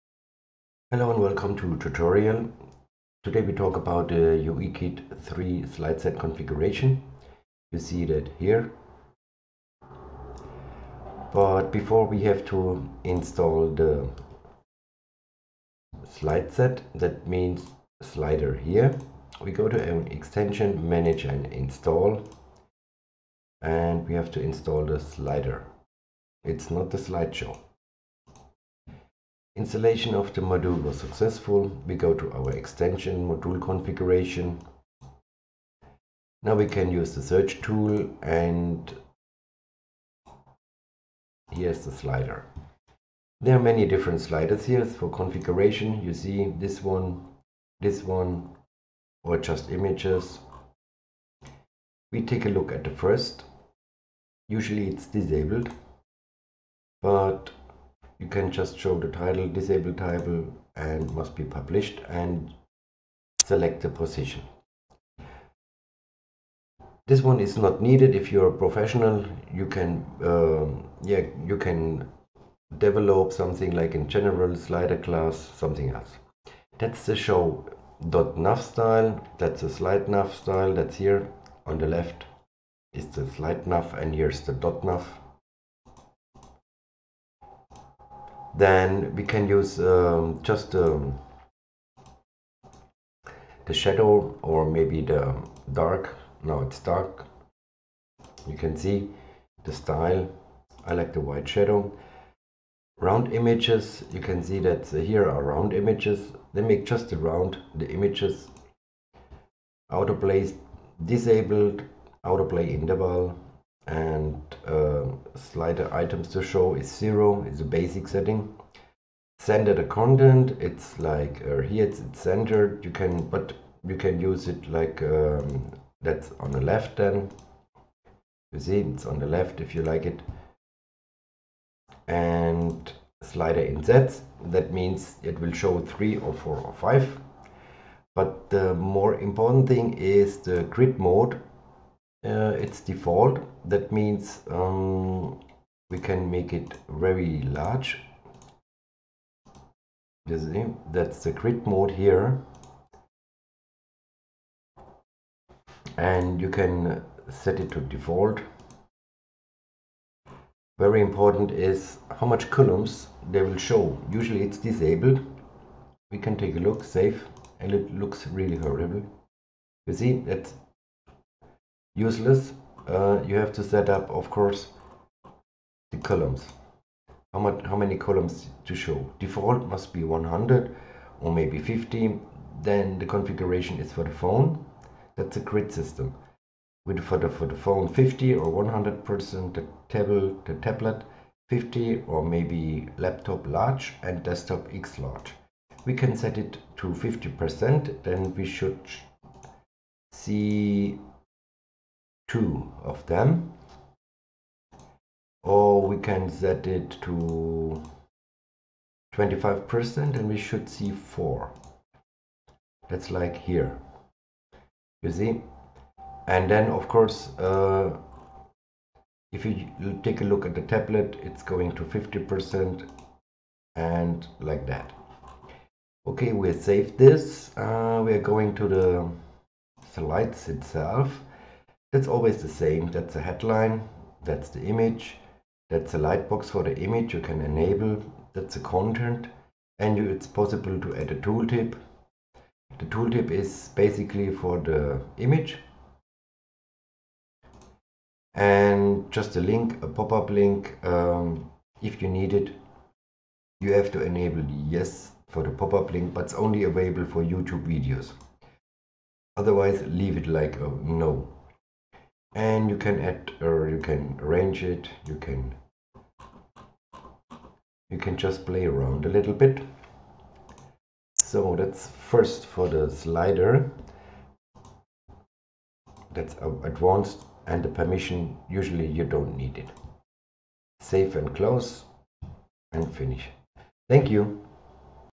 Uikit 3 Slider Module
The Slider component is fully responsive and supports touch and swipe navigation as well as mouse drag for desktops. It even accelerates to keep up with your pace when you click through previous and next navigation. All animations are hardware accelerated for a smoother performance.